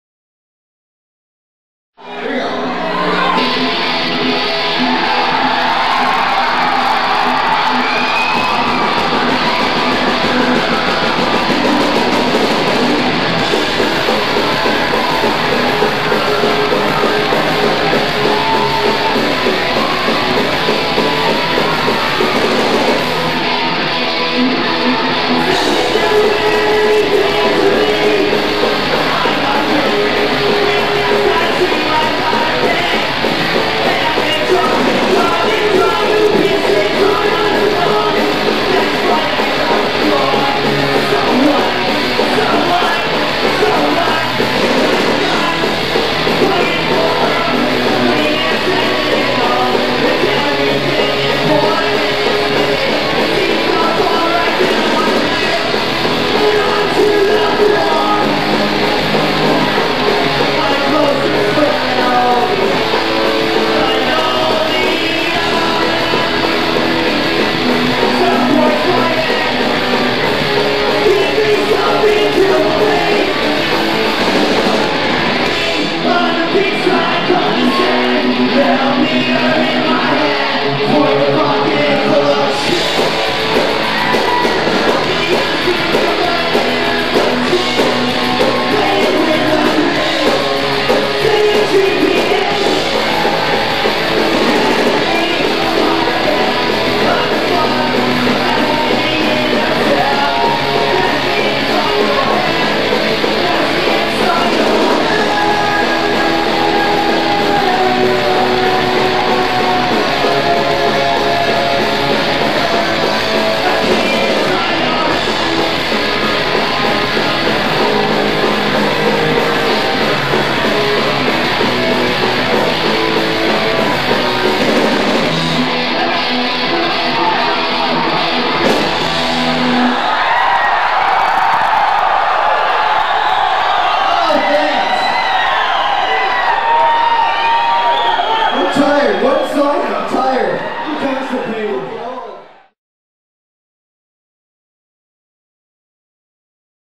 Nofx - Live in Hawaii 1996
*this is just one track... went back and added the rest of the available footage, including soundcheck, in a couple other posts... but wasn't able to get the whole set.

Do you have old tapes of Hawaii shows?
Can handle most tape types as well.